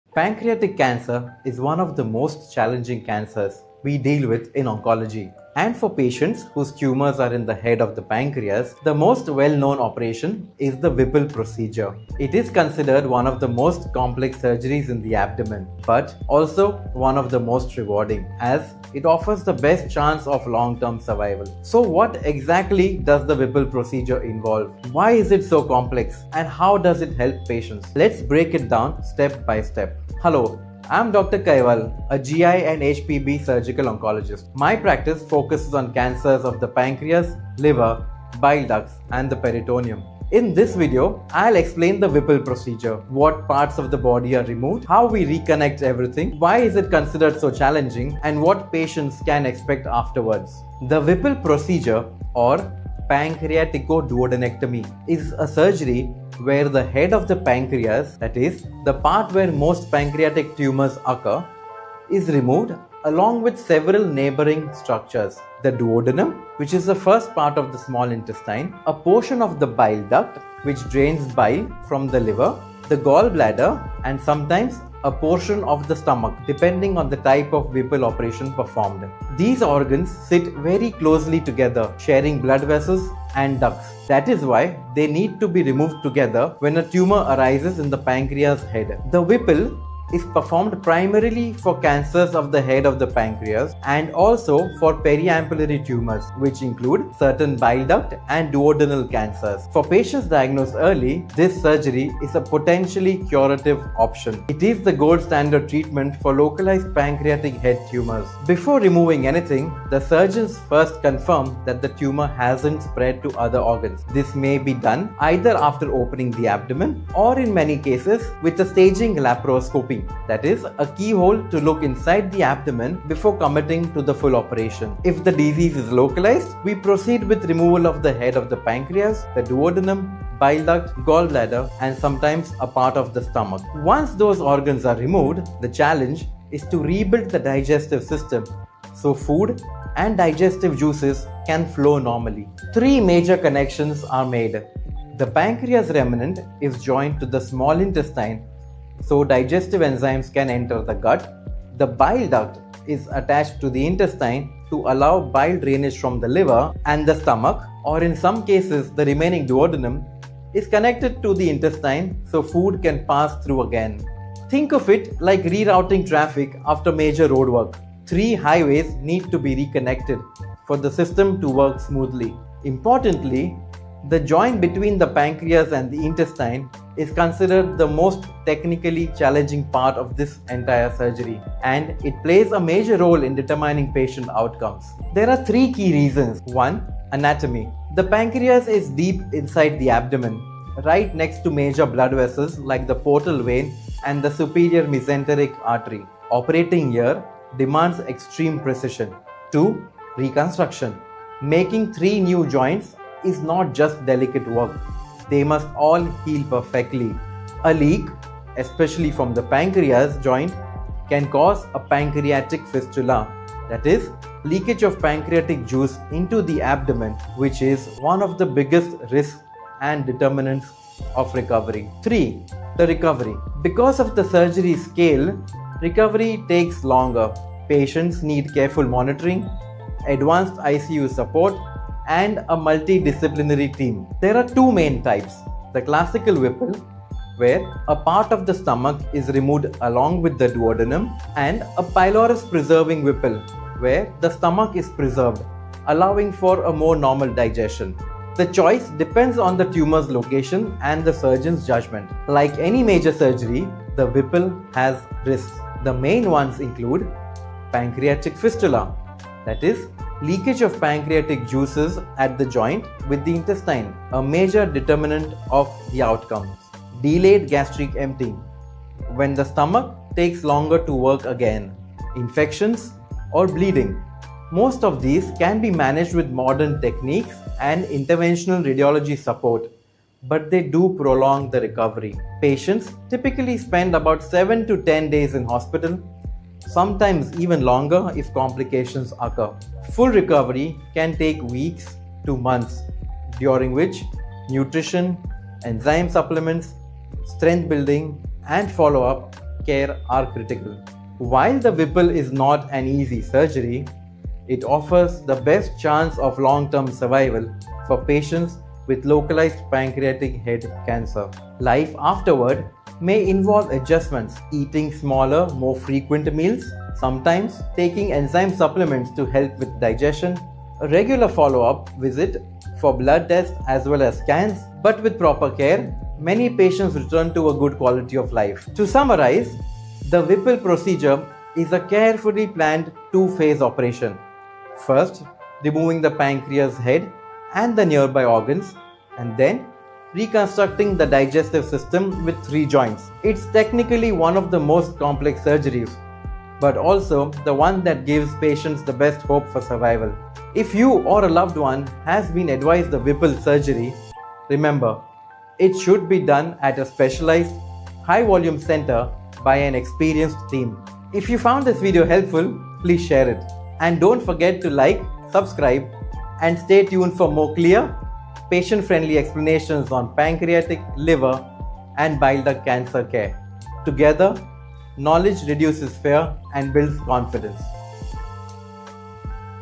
Whipple Procedure Explained Step by Step | Pancreatic Cancer Surgery
Whipple Procedure – Step by Step Explained
The Whipple procedure (pancreaticoduodenectomy) is the most important surgery for tumors of the pancreatic head and periampullary tumors (including bile duct and duodenal cancers). It is one of the most complex abdominal operations, but also one that offers the best chance of long-term survival.
In this video, Dr. Kaival (GI & HPB Onco-Surgeon) explains in plain language:

-What the Whipple procedure involves

-Which organs are removed

-How surgeons rebuild the digestive system

-Why it is considered so complex

-Variations: classical vs pylorus-preserving

-Risks, complications, and recovery

-Life after Whipple surgery

✅ Patient takeaway: The Whipple procedure is challenging but life-extending. Understanding the steps can help patients and families feel more confident and informed.

[Whipple surgery, pancreaticoduodenectomy explained, pancreatic cancer treatment, periampullary tumor surgery, pancreatic head cancer, HPB oncology, GI cancer education]